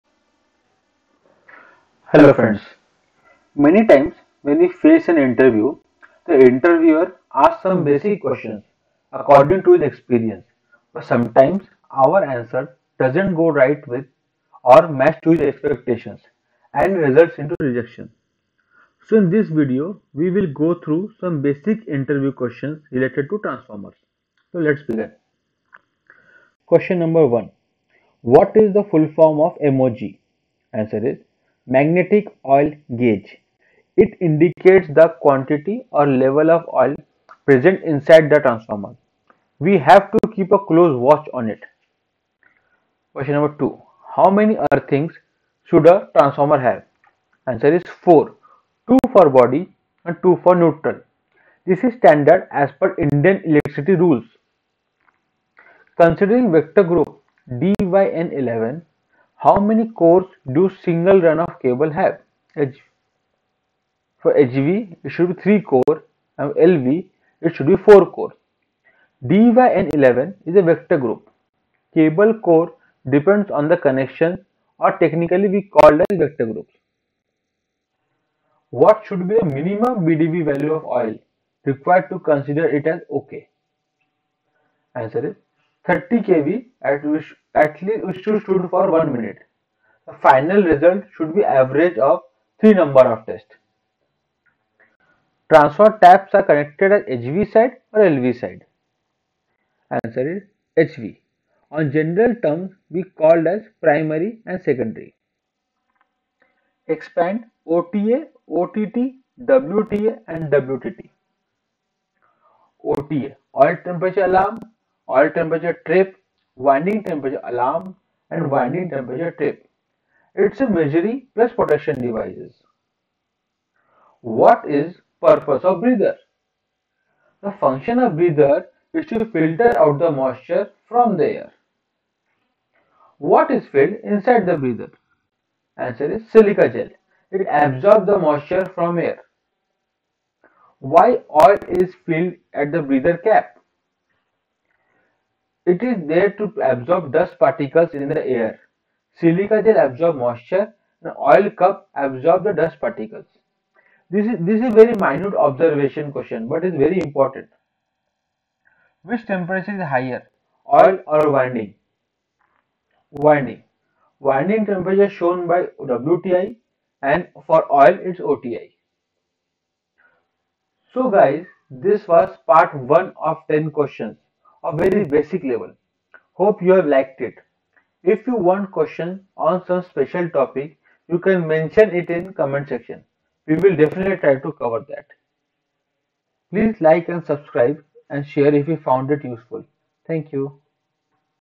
Transformer Interview Question - Quick knowledge
A very quick knowledge on transformer basic questions for an interview.

You can brush up or test your transformer knowledge.